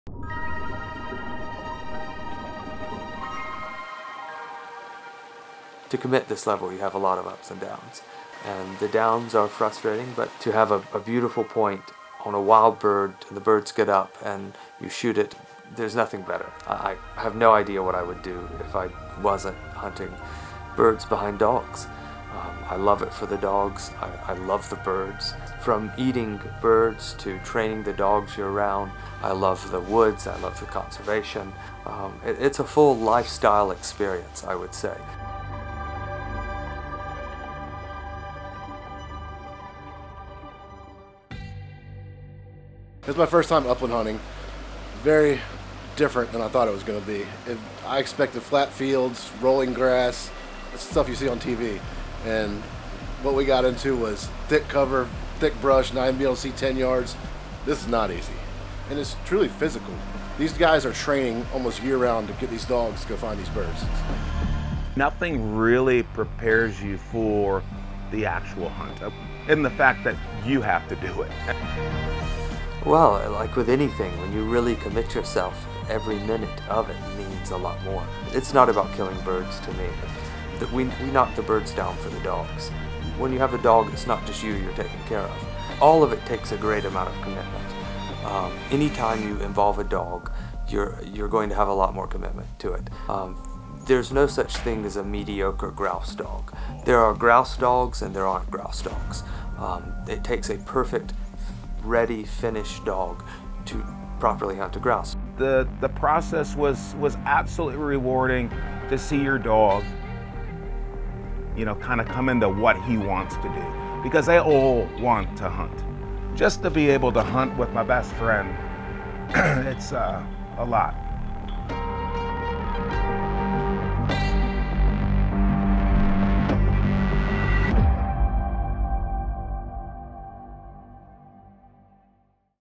What I LOVE about Upland Bird Hunting.
Zane, Brad, and Justin set out into the New Hampshire wilderness to explore a new landscape of bird hunting for the crew, one that is stark in contrast from that of VA public land and preserve hunting. Oceans East Timber & Tines division presents a look into the upland bird hunting trip Oceans East Embarked on in October of 2021.